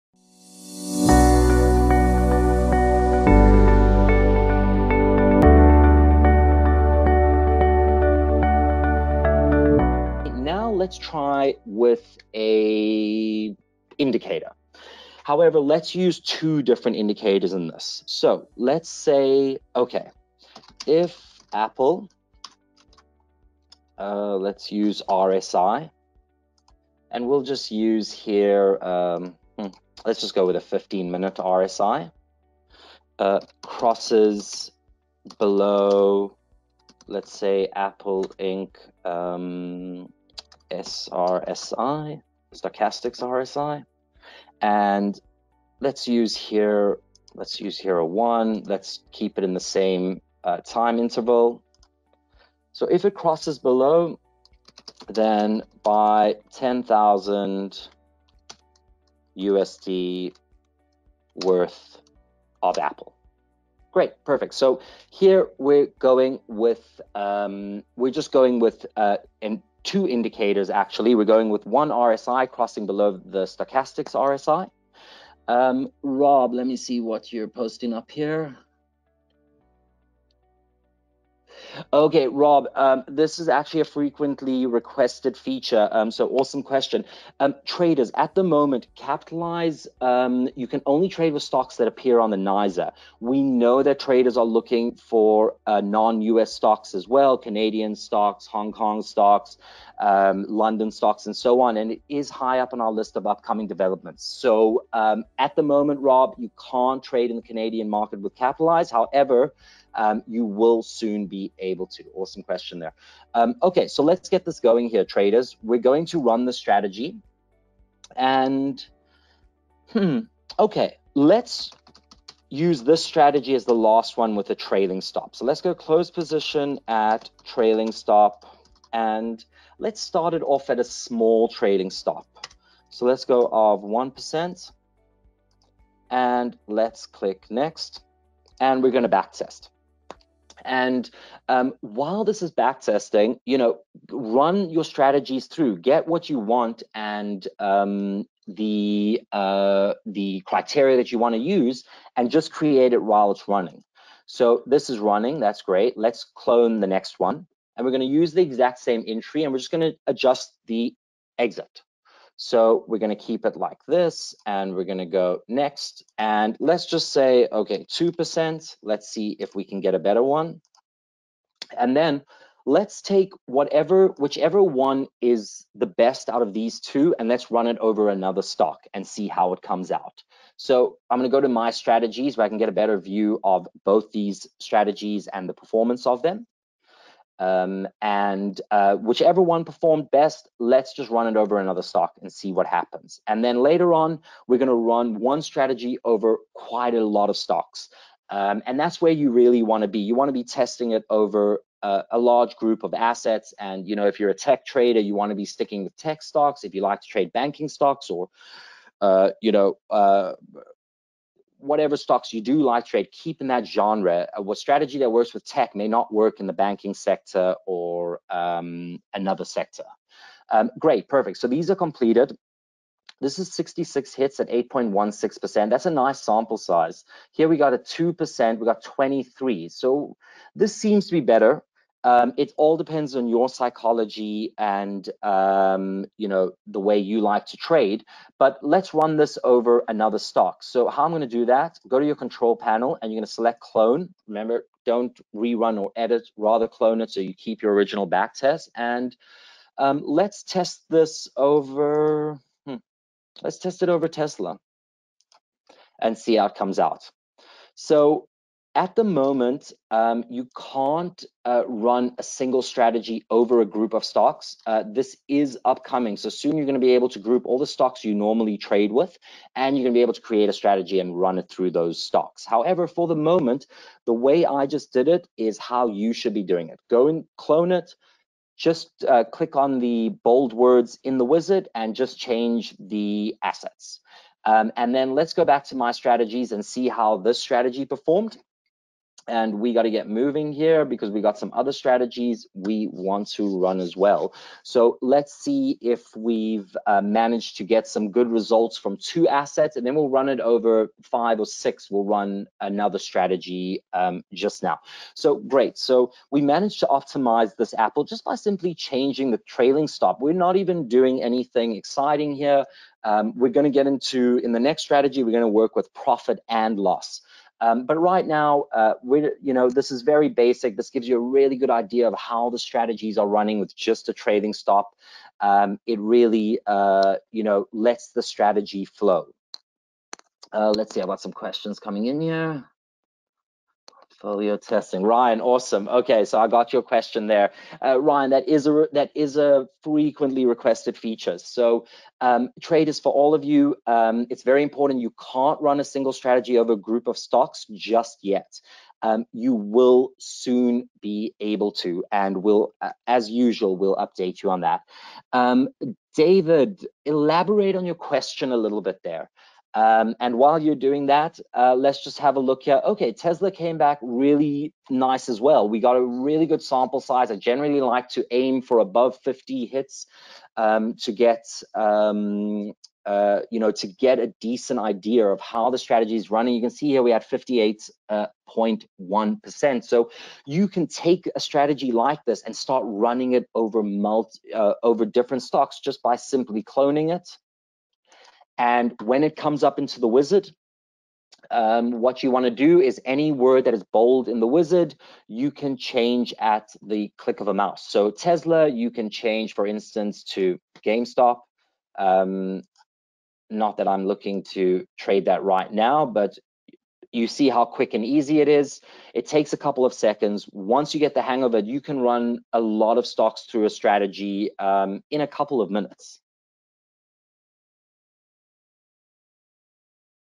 Backtesting and optimizing an RSI/SRSI crossing trading strategy over a group of stocks
In this snippet from our webinar for Interactive-Brokers traders, we demonstrate how to test & optimize a trading strategy in which the RSI crosses below the SRSI and compare its performance over AAPL vs. TSLA.
In a few moments we already got the results and were able to compare & analyze the strategy performance.
Watch and learn how easy it is to test trading strategy and refine it to improve your return rates.
Plan like a human. Trade like a machine